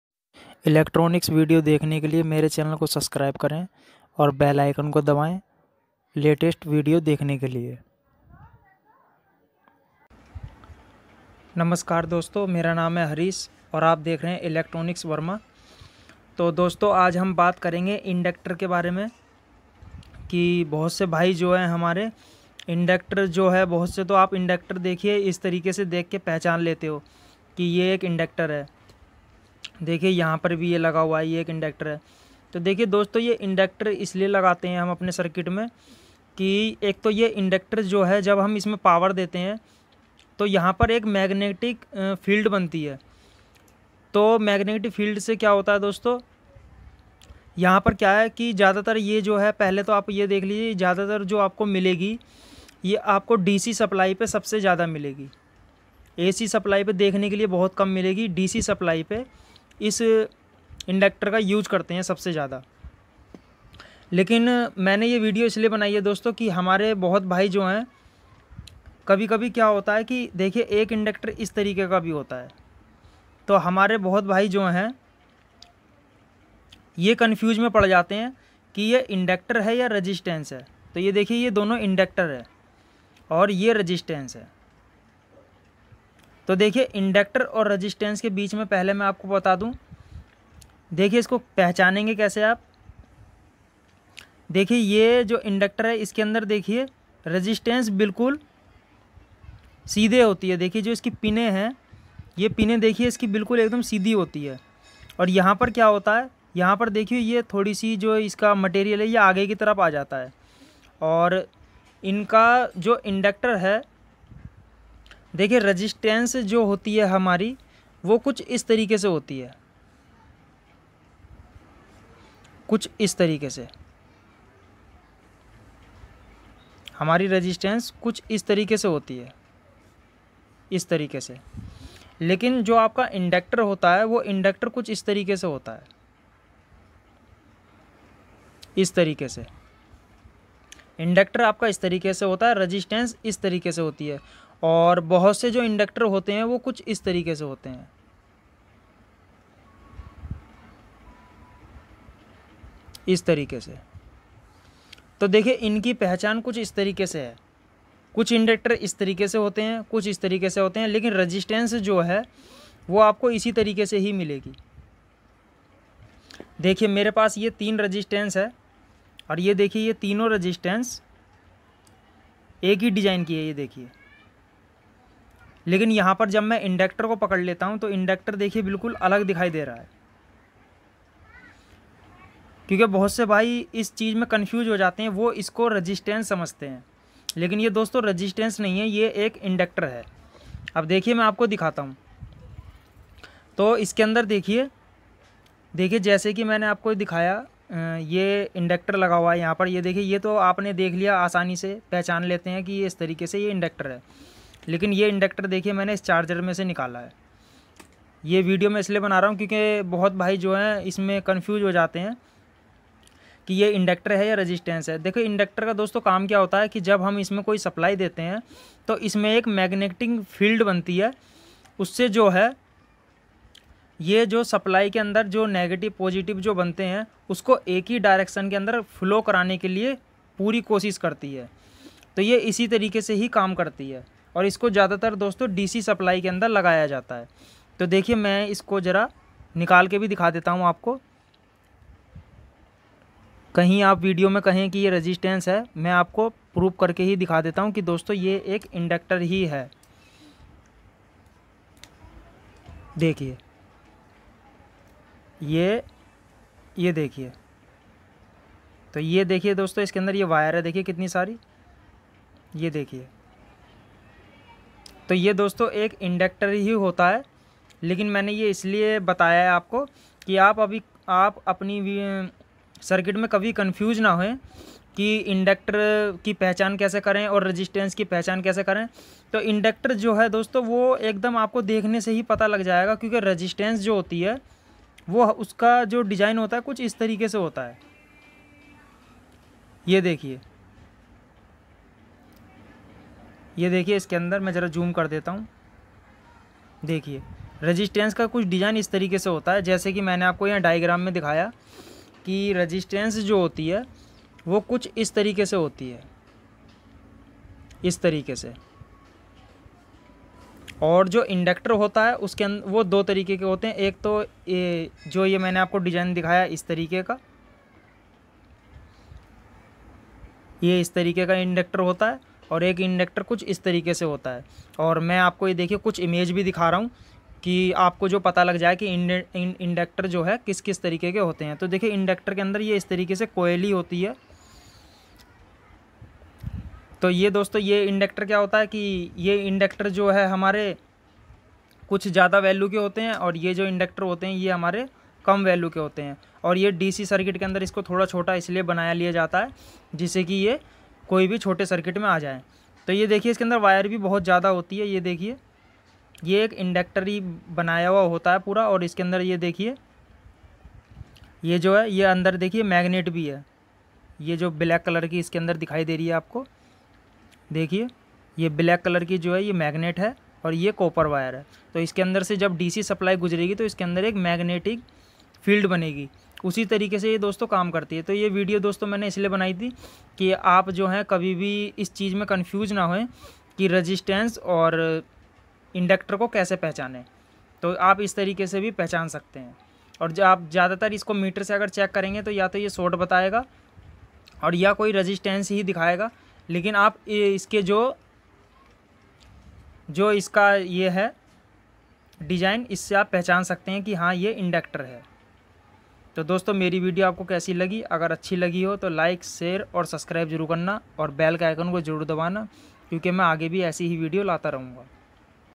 inductor working in hindi || rasistar inductor केसे पता करे || inductors and inductance
rasistar indicator pata kare ..inductor and magnetic field jam karta hai..